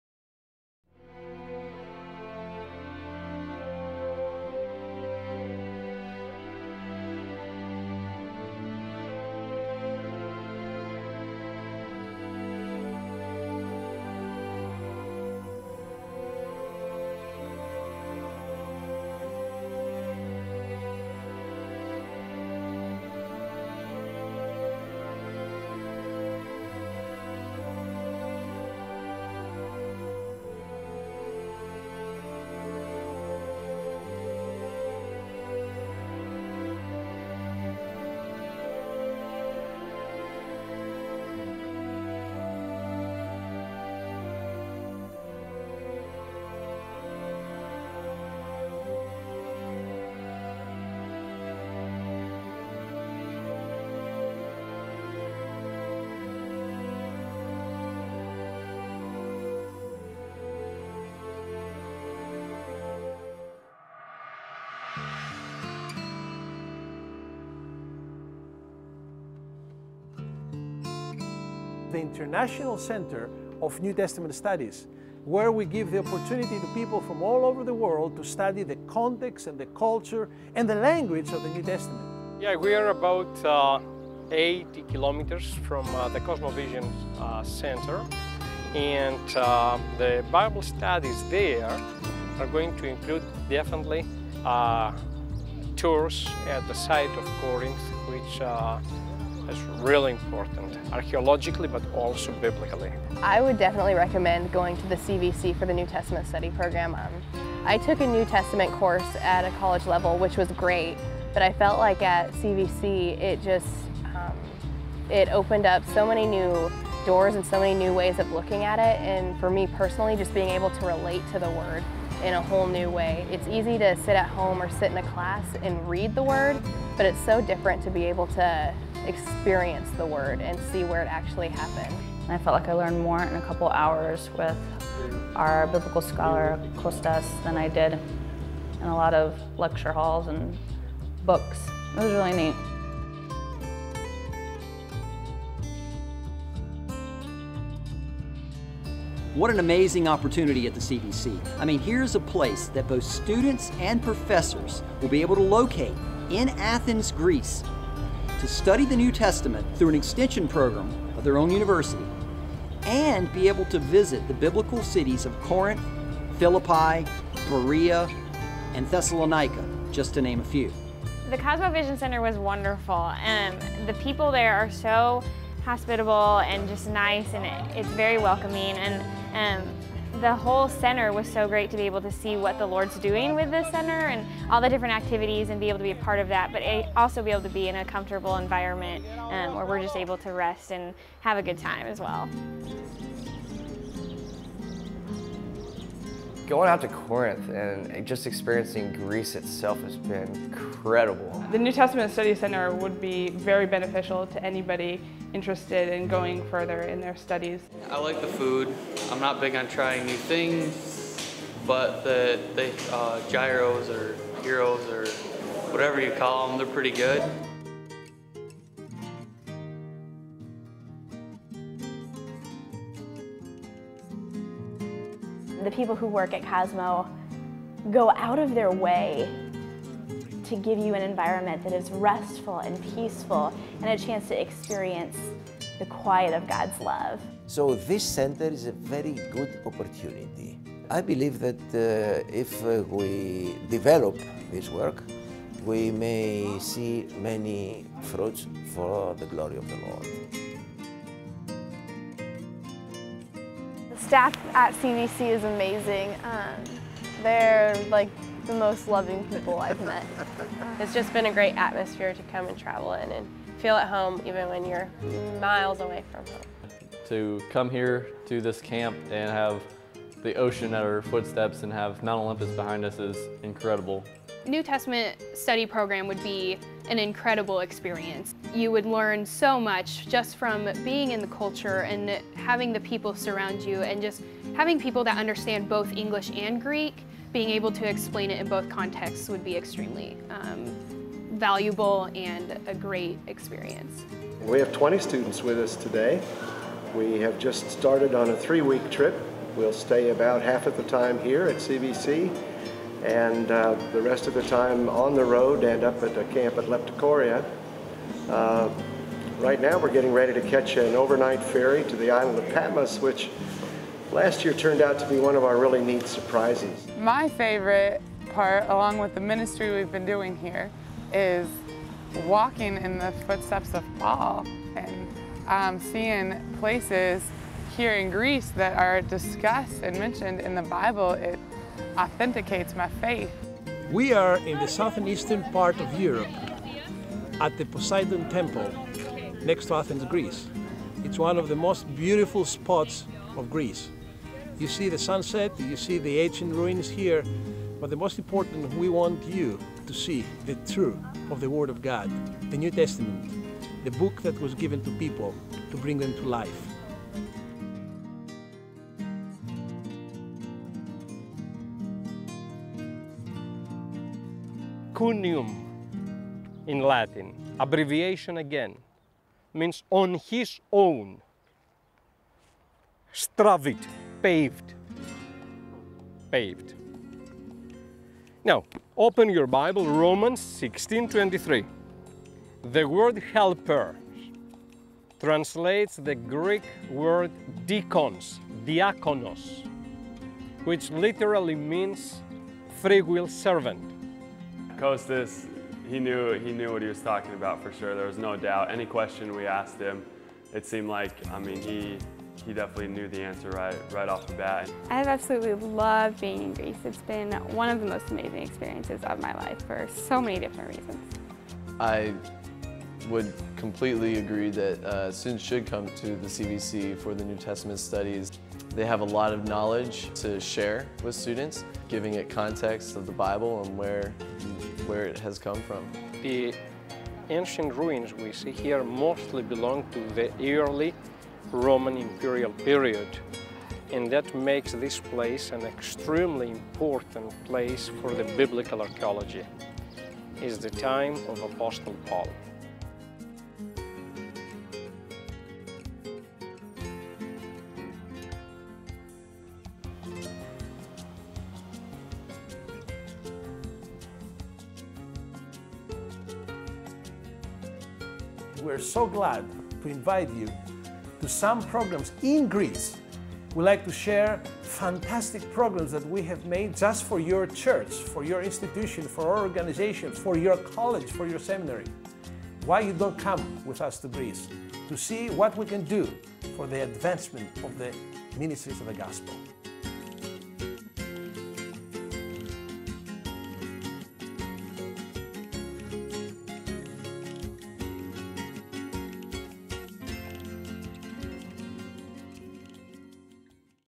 Zodhiates International New Testament Studies Center
A ministry of the Cosmovision Center, the Zodhiates International New Testament Studies Center is operated at the new Bob and Joan Mallory campus. The Zodhiates Center has grown out of years of ministry to US college students through their institution's Bible and Missions departments. Now, students and their professors will be able to locate in Athens, Greece to study the New Testament and visit the biblical cities of Corinth, Philippi, Berea, and Thessaloniki all within driving distance. Israel, Egypt and Asia Minor (today Turkey) are also easily accessible in the region.

The Zodhiates Center will provide Christian colleges and universities worldwide the unique opportunity to enhance their curriculum through customized extension programs in Greece. God's people from Eastern Europe, the Arab world, and North Africa will also be able to benefit by these high quality Biblical studies, without incurring the associated high costs of studying in North America.

Most recently, students from Master's College in California took advantage of CVC facilities and location. Their 21-day trip began in Antioch, continued through Asia Minor and the churches mentioned in Revelation, moved on to Philippi, Thessalonica, Corinth, and to CVC in Athens. This cooperative academic program provided them opportunity to hear challenging messages and testimonies, and to study biblical history and archaeology "on location". Plans are in place for other students from Master's College, and from other US schools, to participate in similar programs.